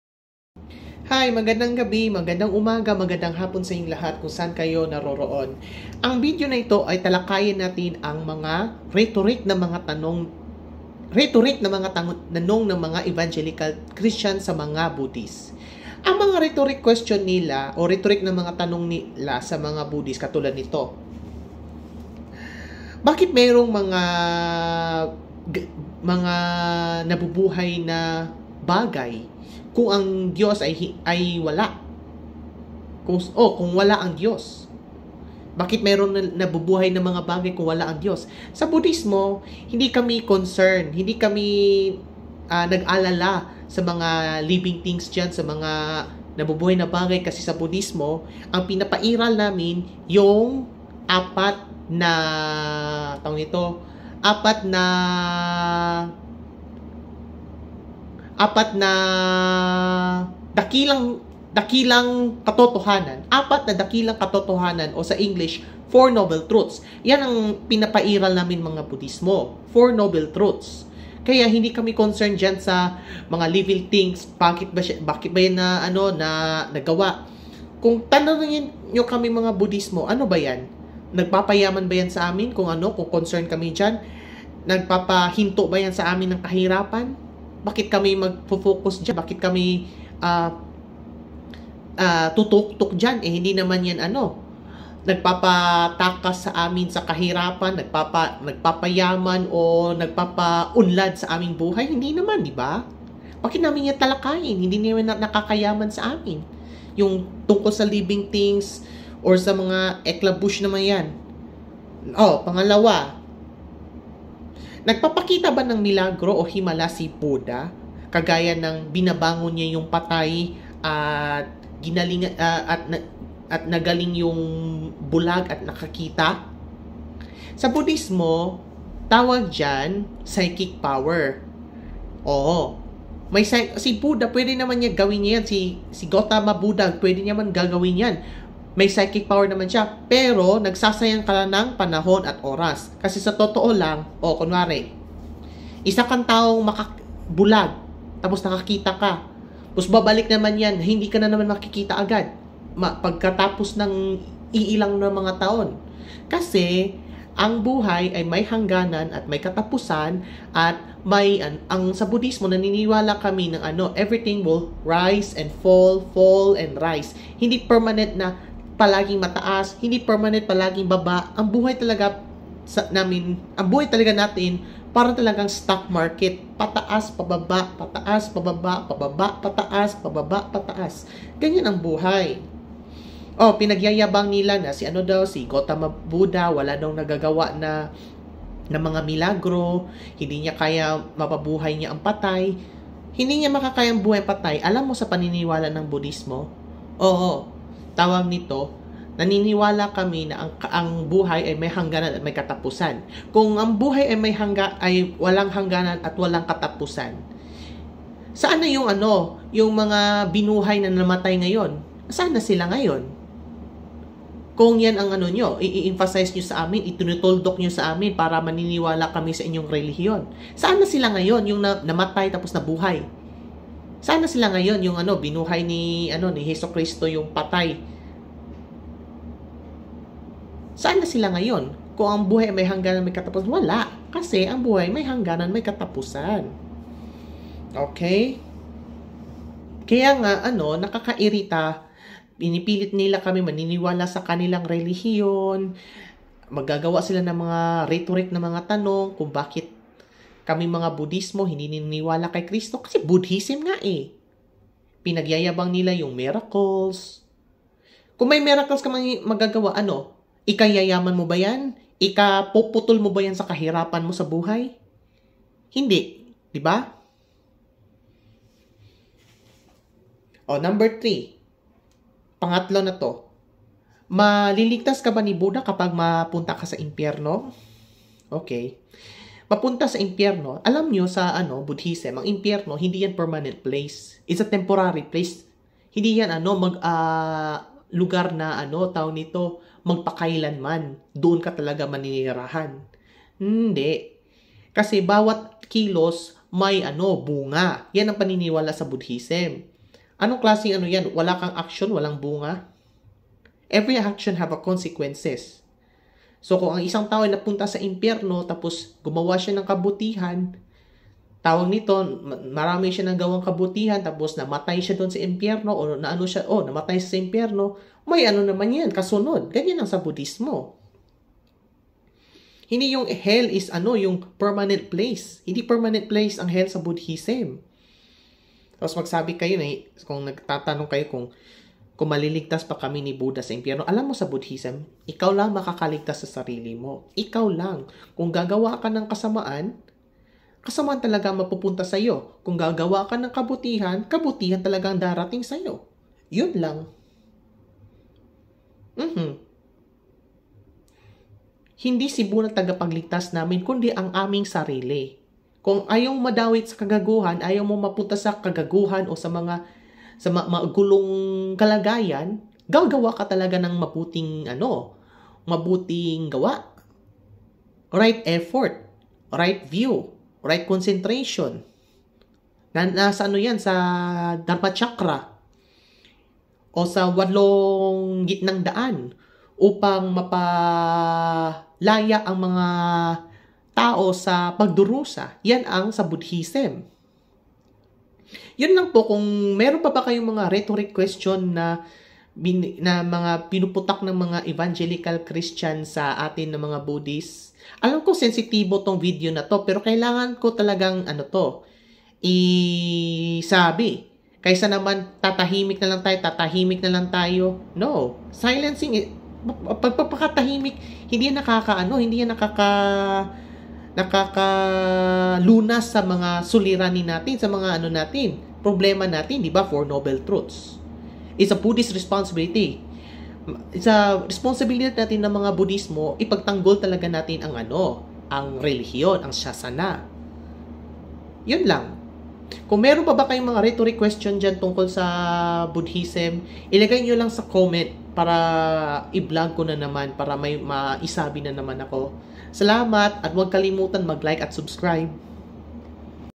Mga Rhetoric na mga tanong ng mga Evangelical Christian sa mga Buddhist.
Nakakapagod parang nakakaasar na ang kanilang kilos kaya nag-gawa ako ng video patungkol diyan.